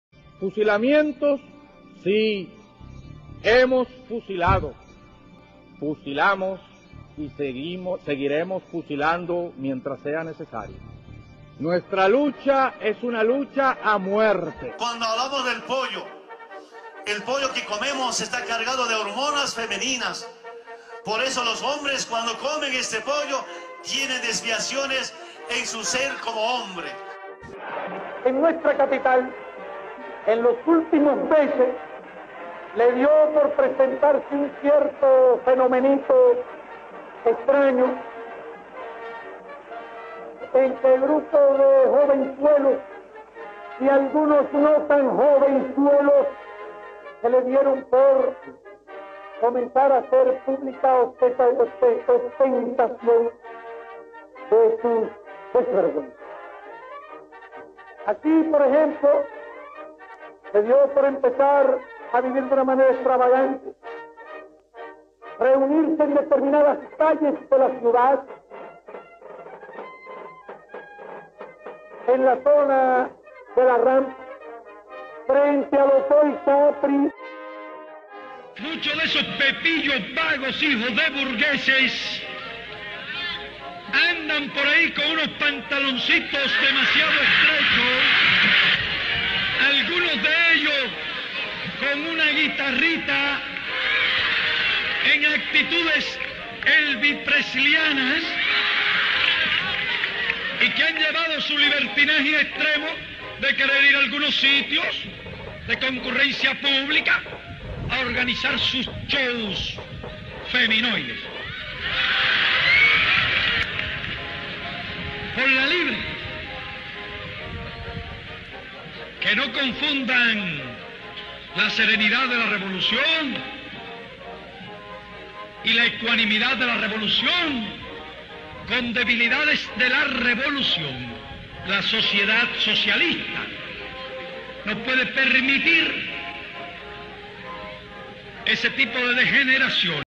Pasen y vean lo que piensan los lideres de la izquierda de la homosexualidad
Hablan el Ché Guevara, Fidel Castro y Evo Morales